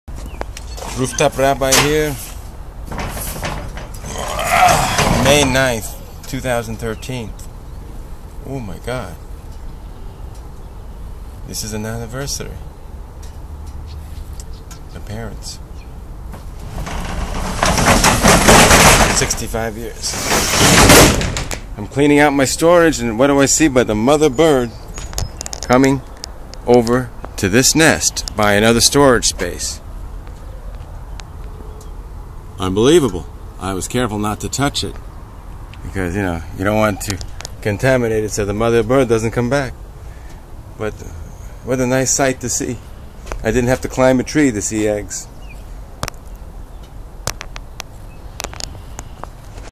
Rooftop Rabbi stumbles upon a robin's nest
While cleaning out his Ohio storage compartment the rooftop Brown by is shocked to find an active Robins nest with four big blue Robin eggs